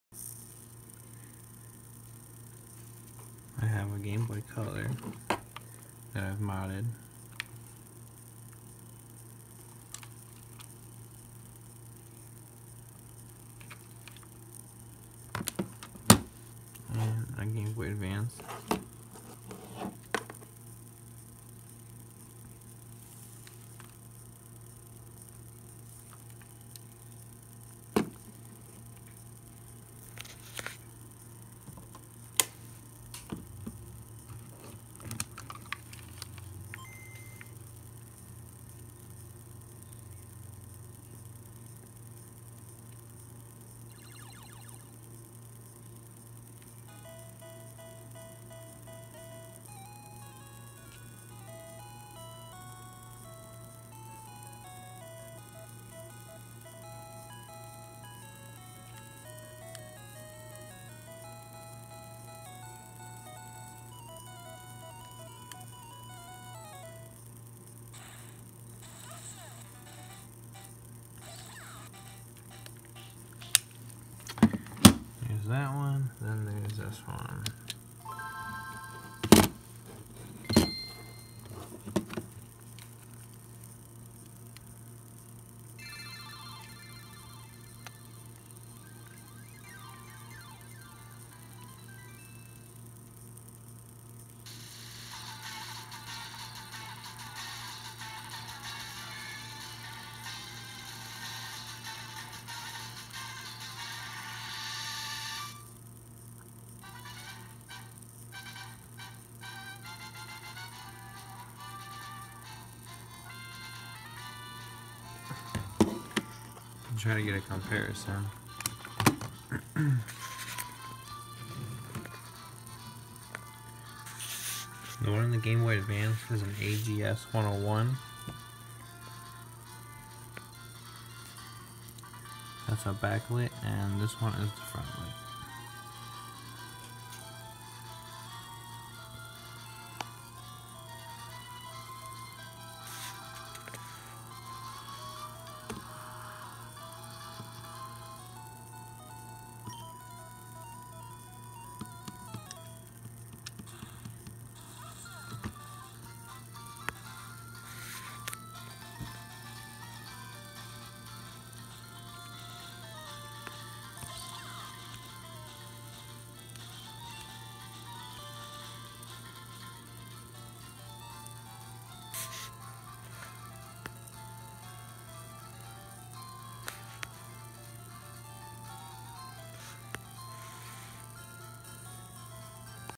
Gameboy mods
Hey guys, just showing what I've done lately. I've done a backlight ags101 screen on my gba, and I've also done a front light mod to my gbc.  Backlight is just way better, but it's nice have a gbc you can play at night if you don't have a gba.  Thanks to handheld legends for the shell and glass screen!